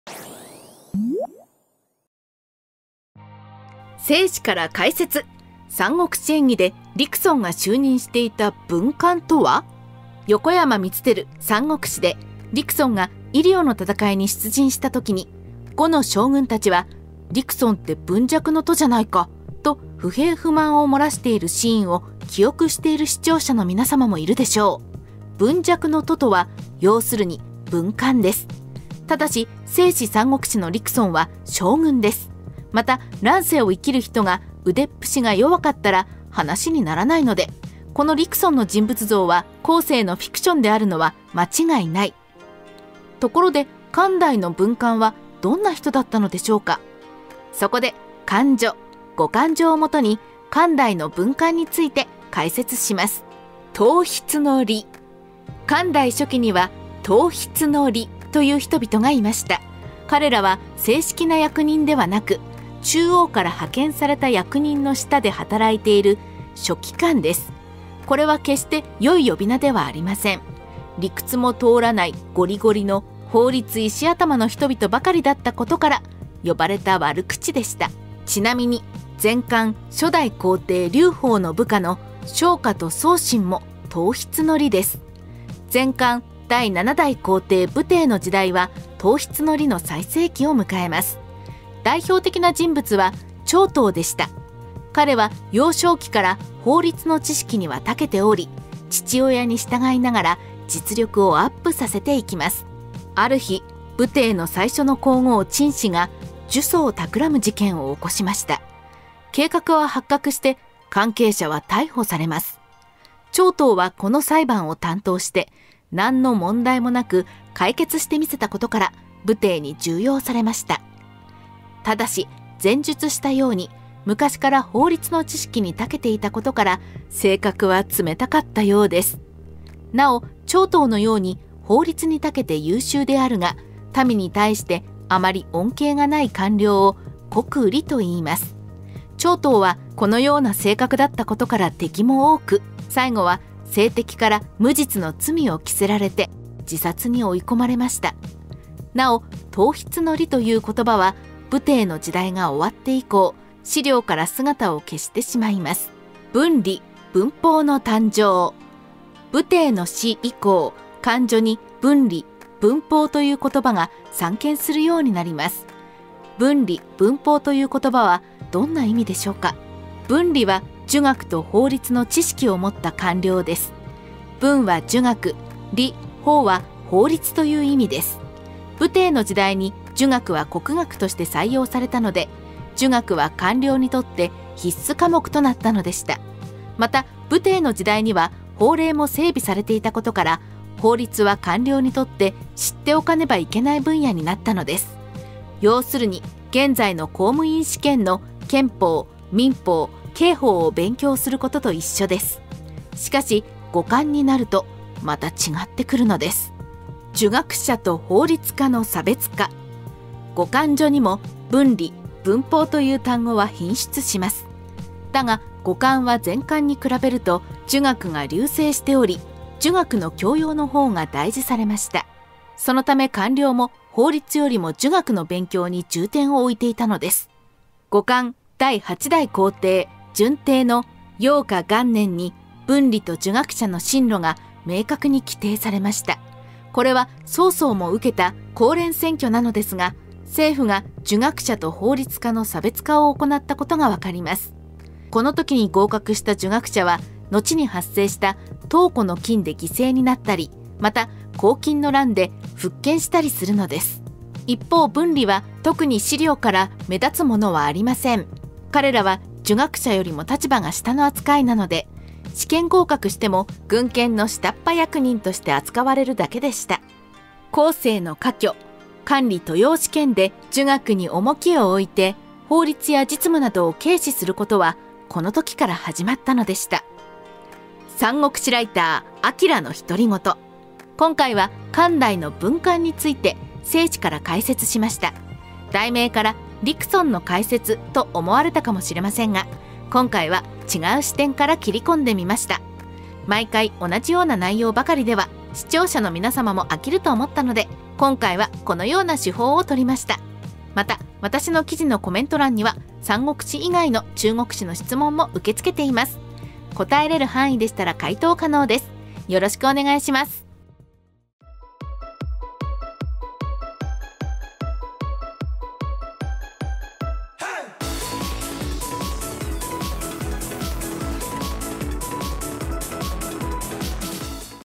文官って何するの？陸遜も就任していた役職
【動画の目次】
文官って何するの？陸遜も就任していた役職 00:00
刀筆の吏   00:54
文吏・文法の誕生   02:37
儒学者と法律家の差別化   03:28
三国志ライター　晃の独り言   04:37

横山光輝『三国志』で陸遜が夷陵の戦いに出陣した時に呉ご（222年～280年）の将軍たちは、「陸遜って文弱の徒じゃないか！」と不平不満をもらしているシーンを記憶している読者の皆様もいるでしょう。あなたは文官についてどう思いますか？

皆様の三国志知識や歴史解釈のコメントお待ちしています！

✅はじめての三国志 Facebookファンページ（短縮URLを表示しています）